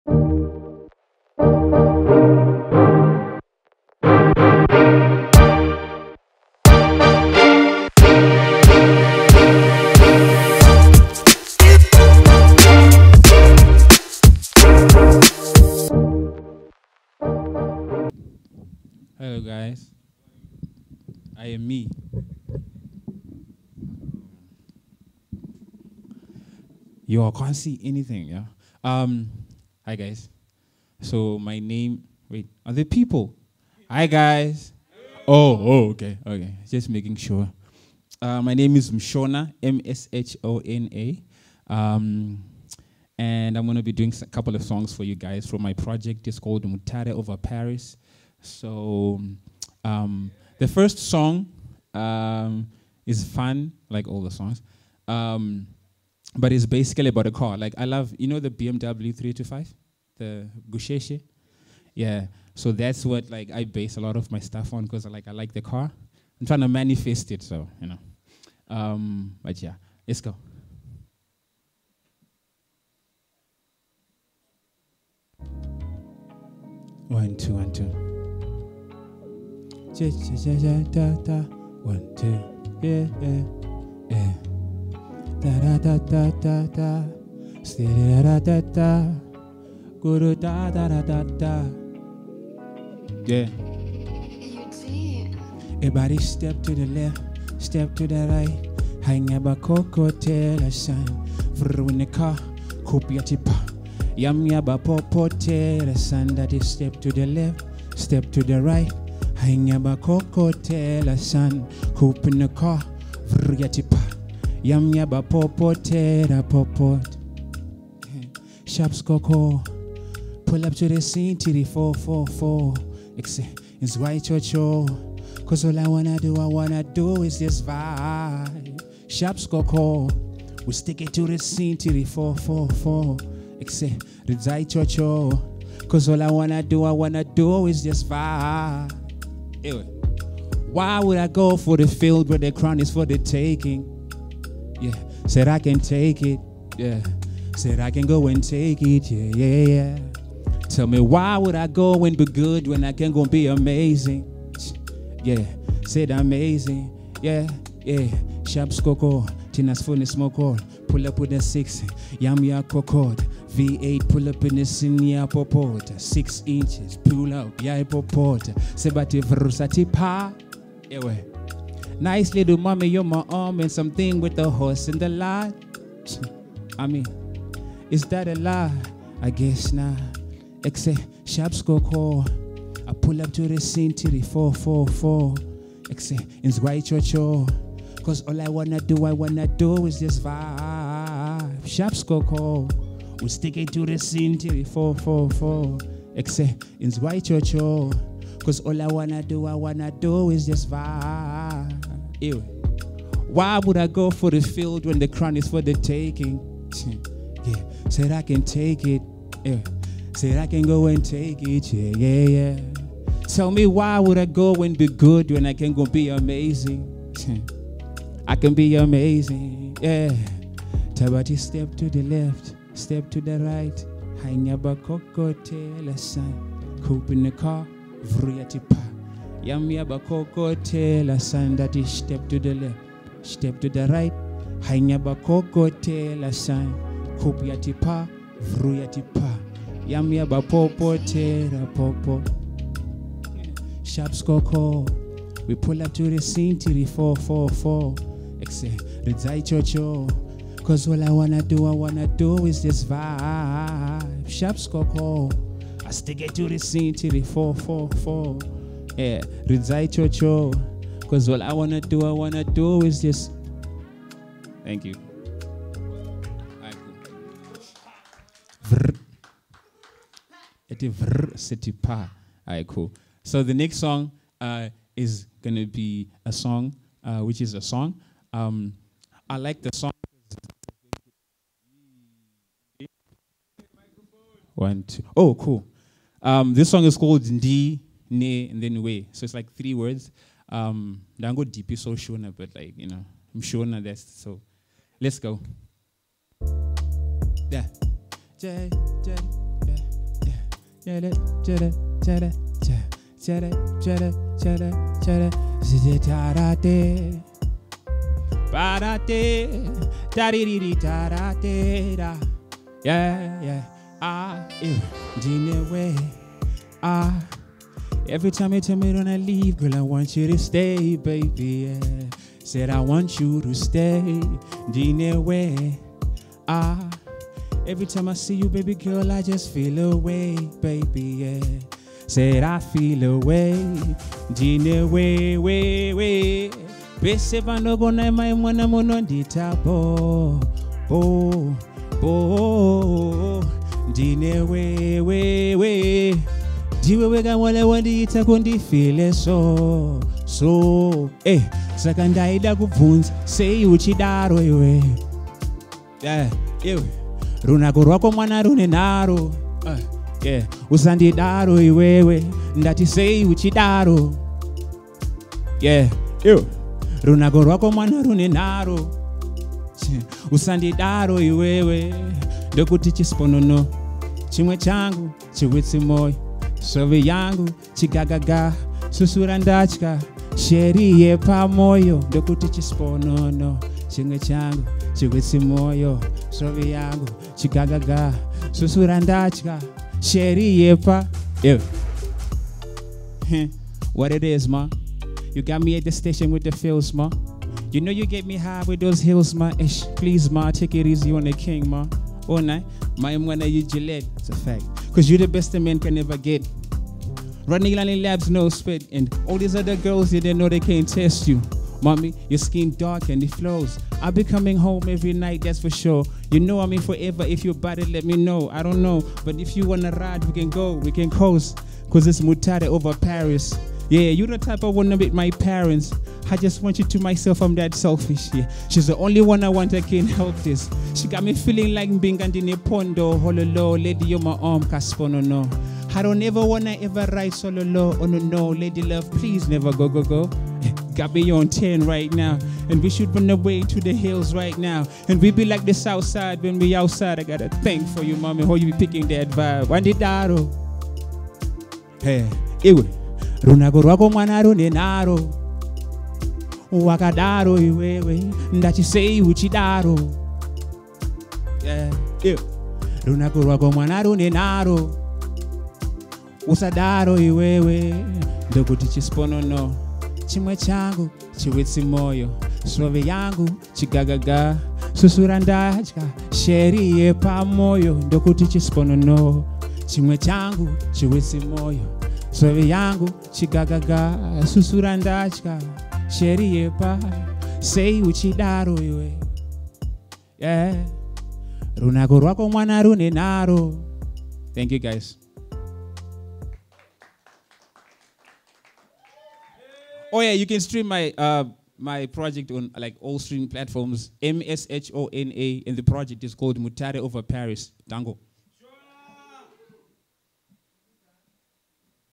Mshona live at the Listening Party (JULY Edition)
The Listening Party is a platform for artists to showcase their experimental work to an audience and receive feedback. Mshona is an amazing rapper, who fuses urban beats with melodic lyrics.

Artist - Mshona
Videography - Uncle Blux
Venue: ZGS/GZH (51 Lawson Avenue Milton Park, Harare)